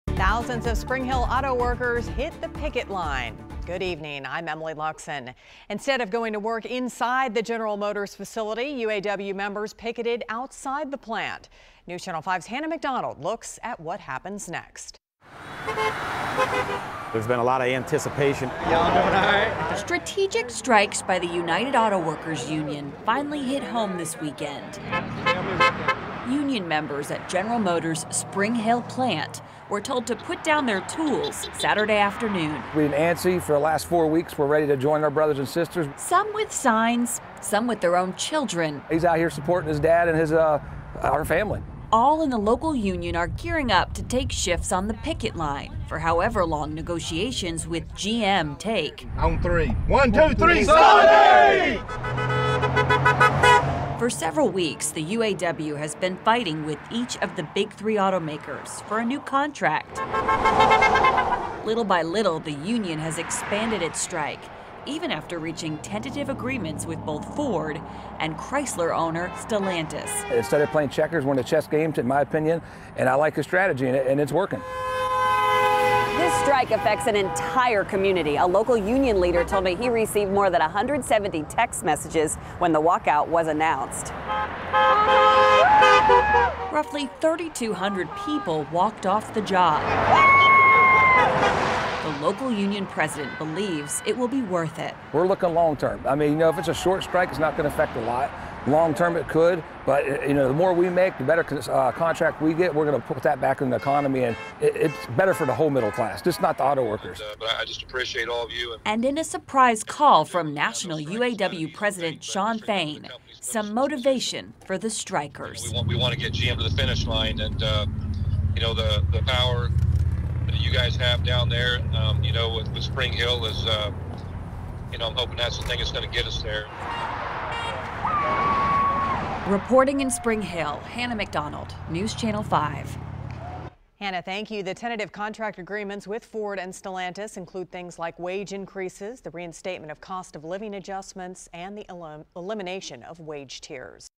UAW orders Spring Hill automakers to join strike against lone holdout GM
Instead of going to work inside the General Motors facility in Spring Hill, thousands of local automakers are now part of the United Auto Workers strike.